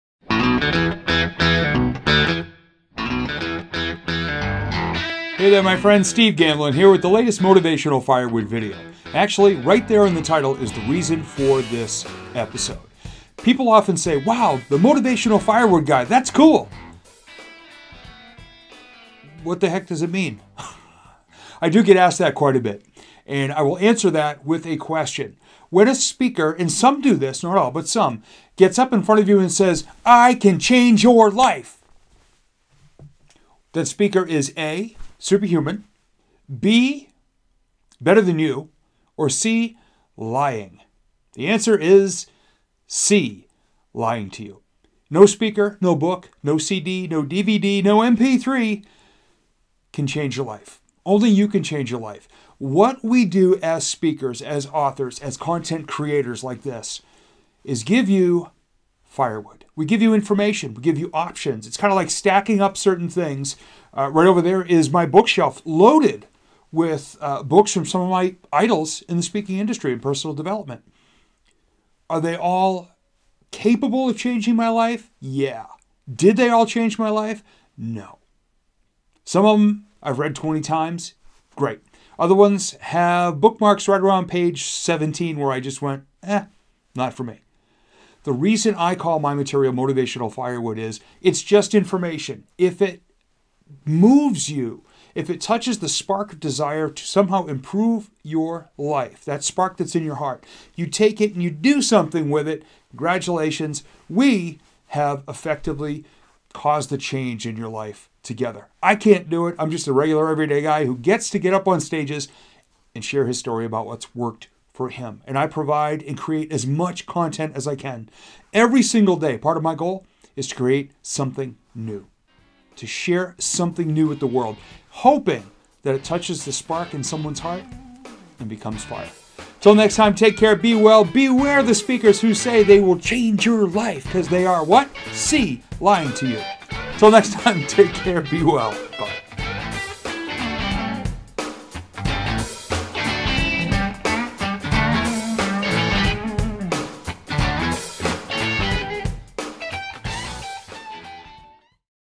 What is Motivational Firewood™ (and are Motivational Speakers Lying to You)?
Too many speakers SAY they can 'change your life'. I disagree.
Here is MY approach to being a motivational speaker and author.
I believe speakers need to walk their talk, live with integrity, be the product of their own learning...and not screw people over by selling them garbage.
All we can do is share INFORMATION. What people do with it to improve their lives, and make changes, is up to them.

Music supplied by Getty Images.